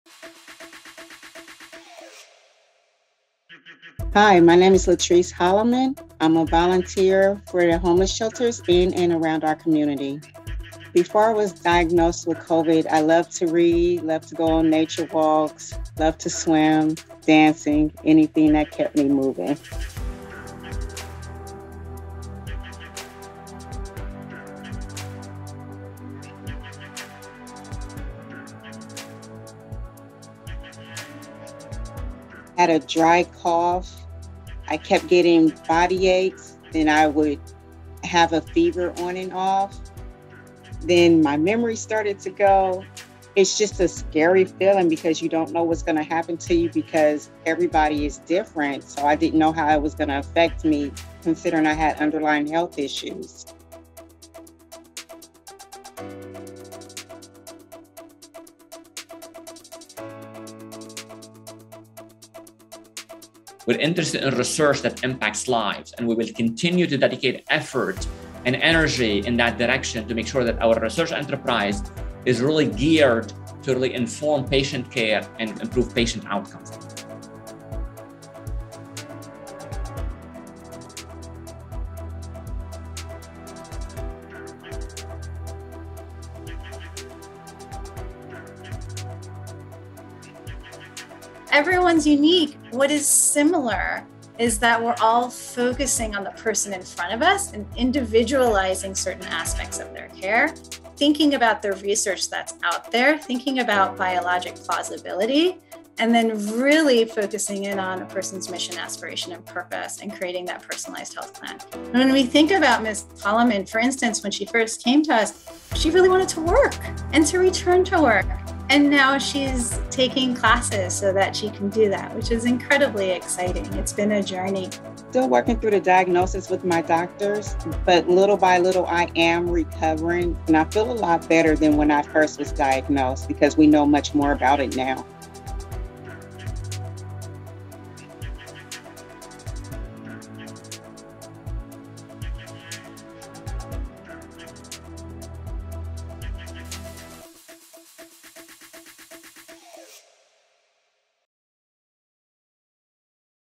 VADidThat: Applying Research to Help Veterans with Long COVID
Veteran LaTrice Hollomon had an acute COVID infection in late 2020. By February 2021, her symptoms were still present and that's when she was diagnosed with Long COVID. Fortunately for Ms. Hollomon and other Veterans who are diagnosed with Long COVID, the Veterans Health Administration is laser focused on researching, understanding, and creating personalized care plans for Veterans with this diagnosis. And the prognosis is promising for the future of COVID care at VA.

To learn more about VHA's Long COVID research, including how to get help, visit this